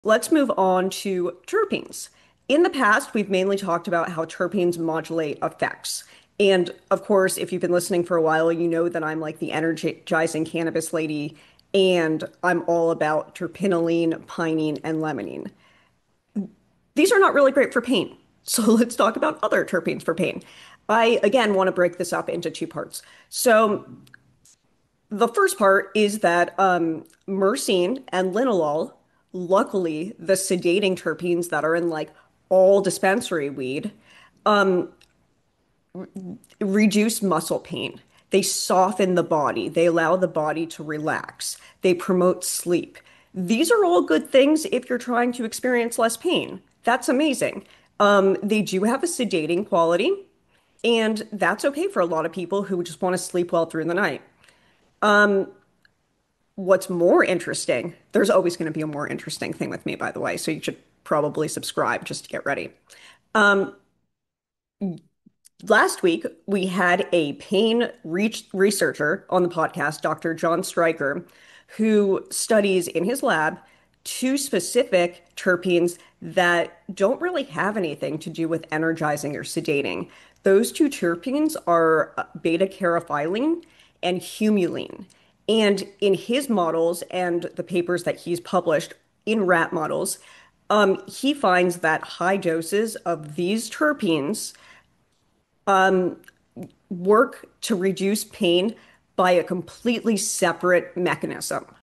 The Terpenes That Reduce Pain
Not all terpenes work the same way — and not all pain relief has to mean feeling sedated.

In this short, I break down the terpenes that are actually associated with pain relief, including why common sedating terpenes like myrcene and linalool can help soften the body — and why two lesser-known terpenes, beta-caryophyllene and humulene, are especially interesting.

Recent research suggests these terpenes may reduce pain through a completely different mechanism than sedation, which matters if you’re looking for relief without feeling knocked out.

If you care about using cannabis intentionally — especially for pain — understanding terpenes is key.